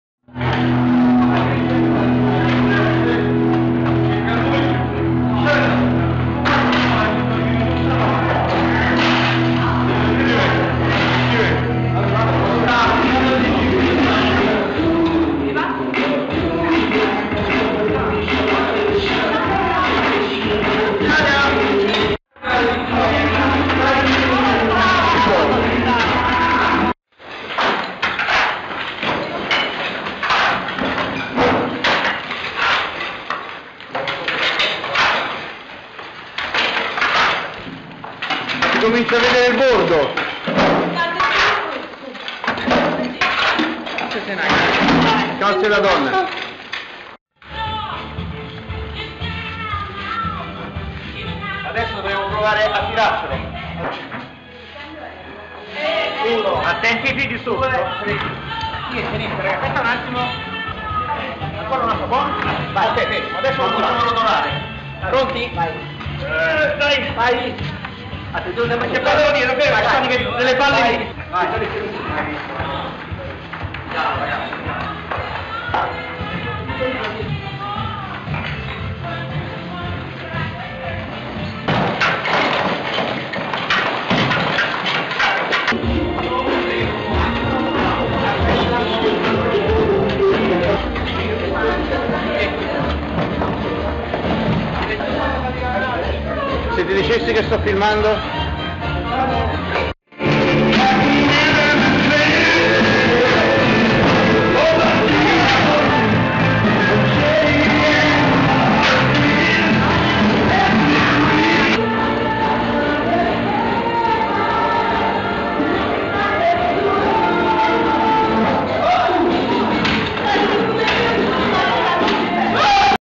Scavi nel Tunnel borbonico
Volontari al lavoro nel Tunnel borbonico. Ritrovata una giara delal prima meta dell'Ottocento (a cura di anna laura de rosa)